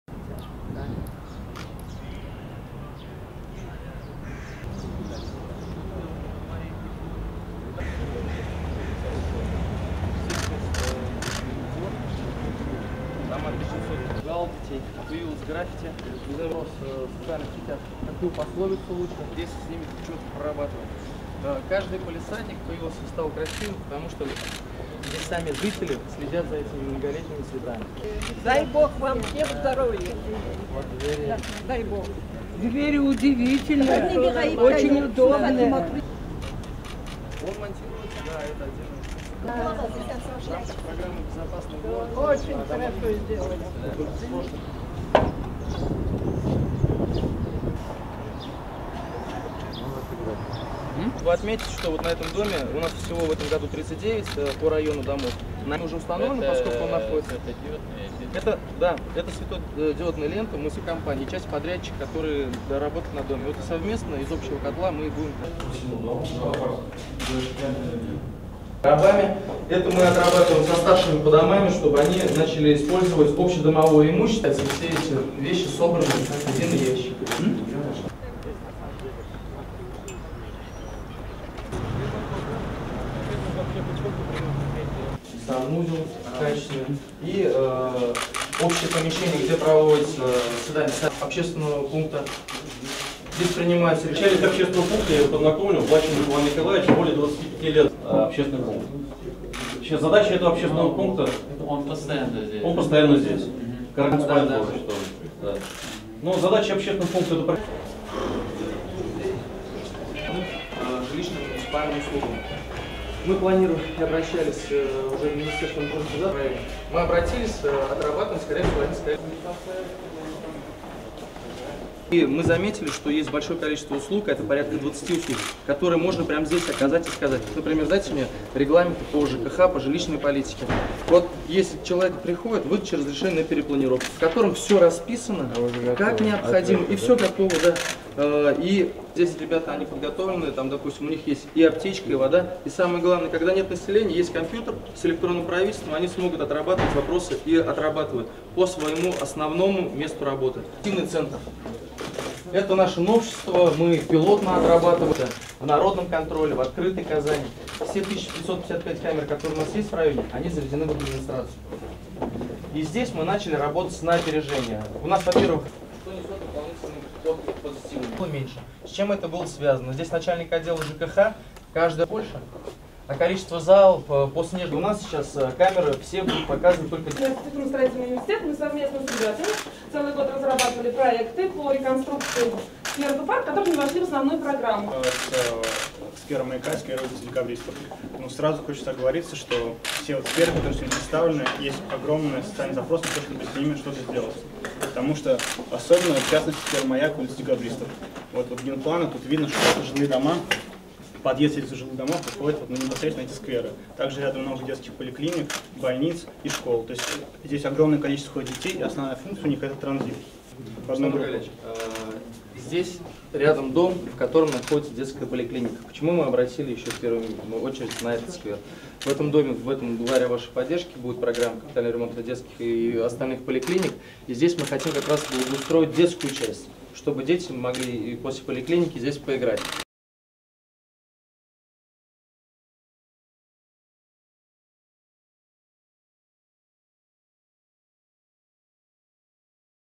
Минниханову показали дома Кировского и Московского районов Казани после капремонта
Президент РТ Рустам Минниханов в сопровождении мэра Казани Ильсура Метшина осмотрел дома Кировского и Московского районов Казани после капремонта. Об этом сообщает пресс-служба президента РТ.